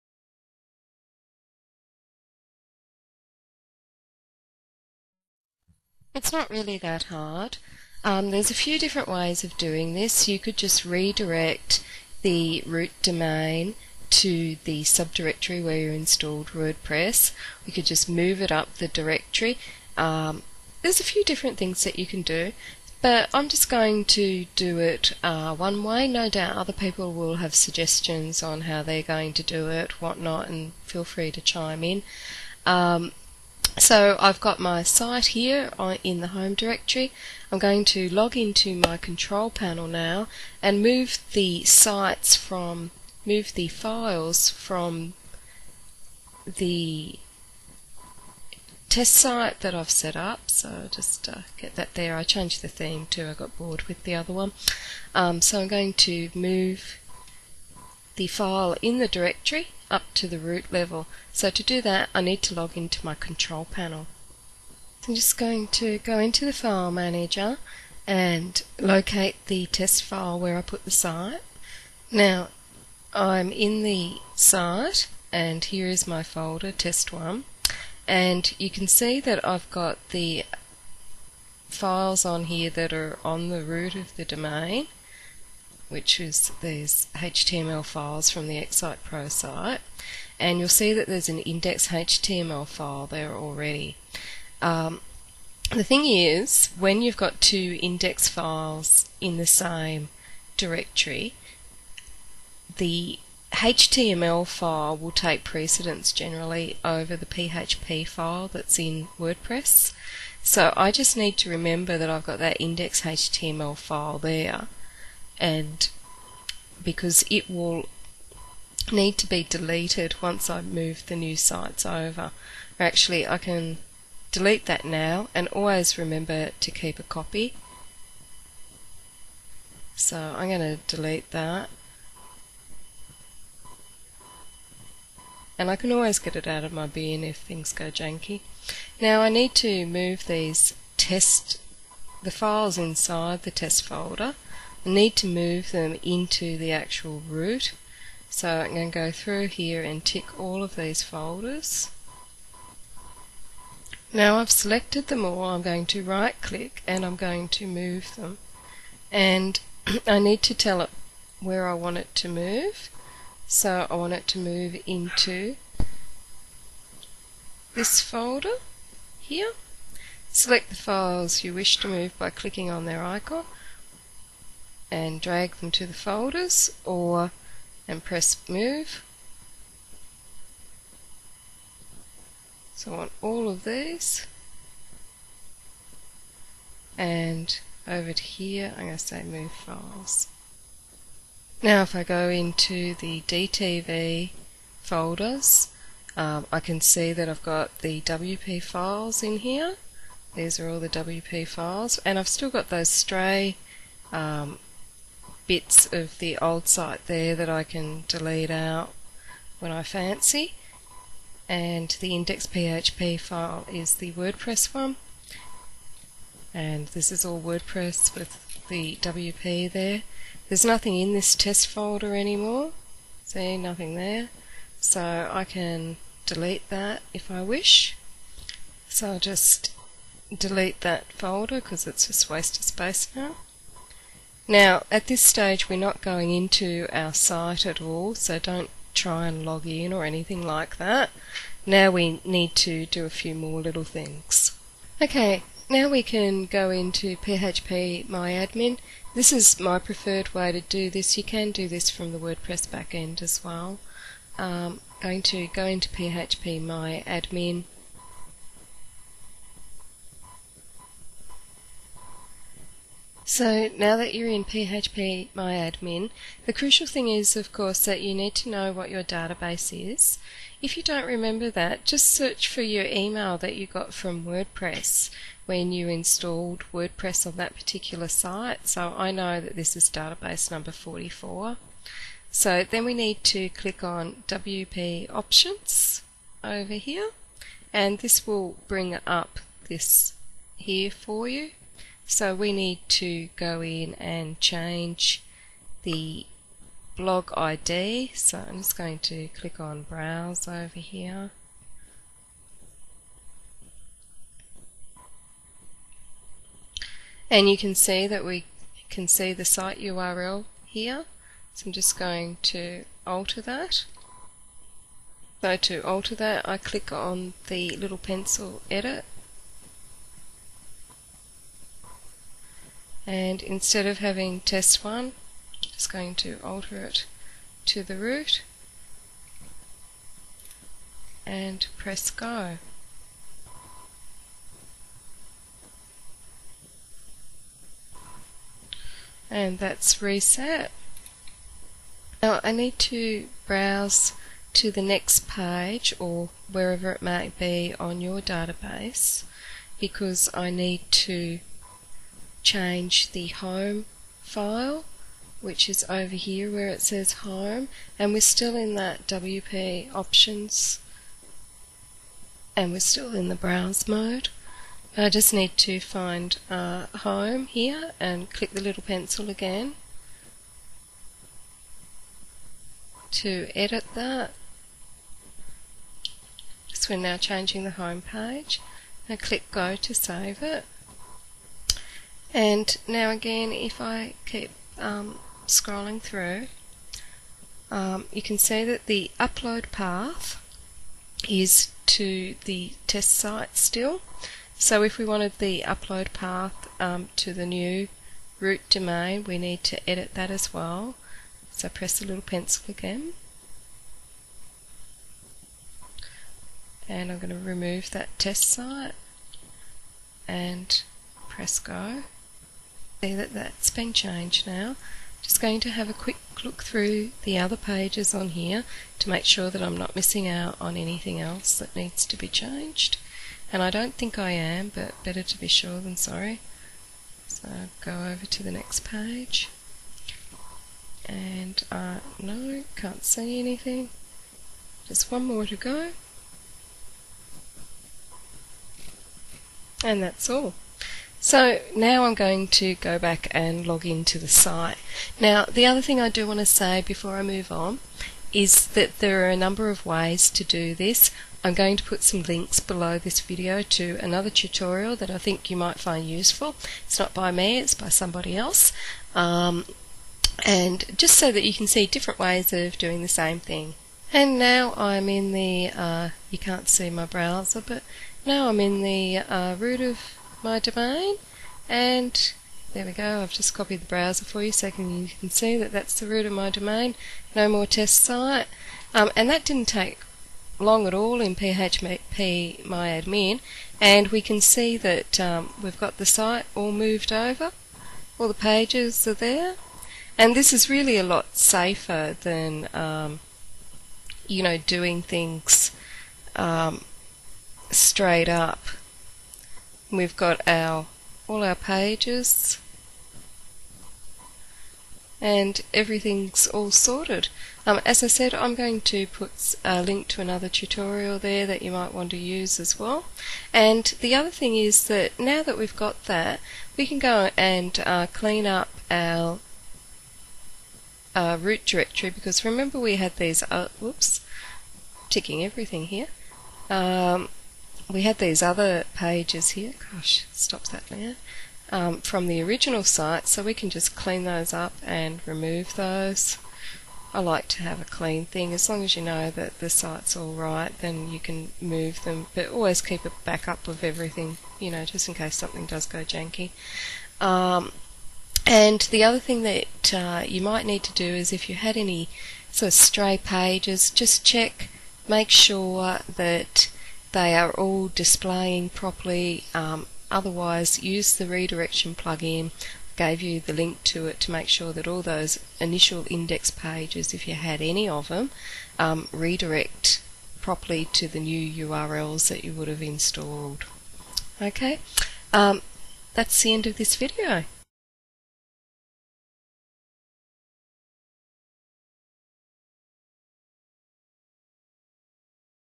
Static Site To WordPress - 3
This is the last video on converting a static site to WordPress.  In this video I will move the WordPress site from a directory to the root of  the domain.